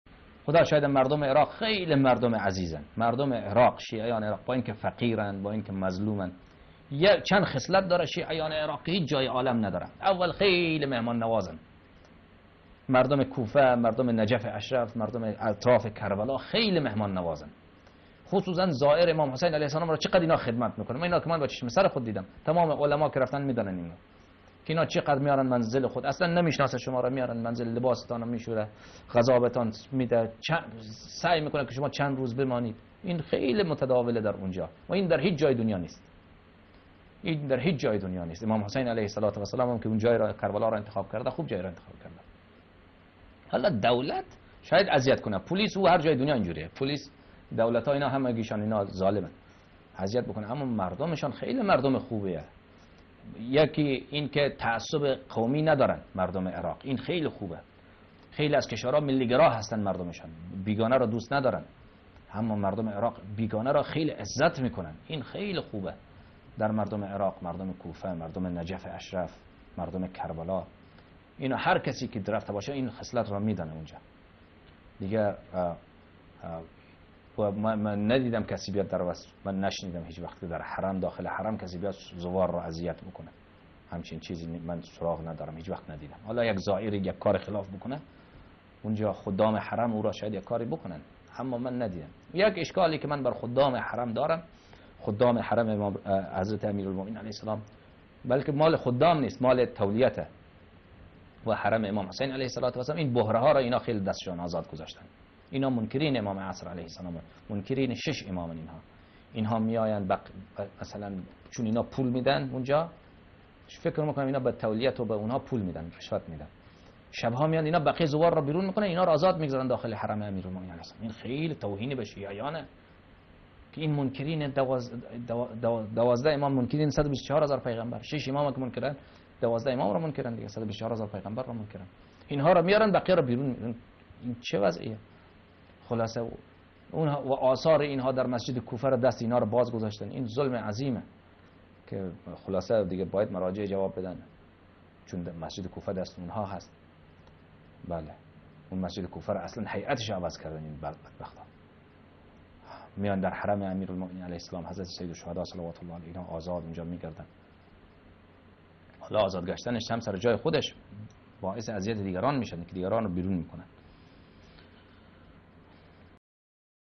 حرمین شریفین عراق شبها دراختیار «بُهره های اسماعیلی»
انتقاد از اخراج شیعیان از حرمین شریفین کربلا و نجف و حضور بهره ها در این اماکن مقدسه
شبکه اهل بیت علیهم السلام علامه اللهیاری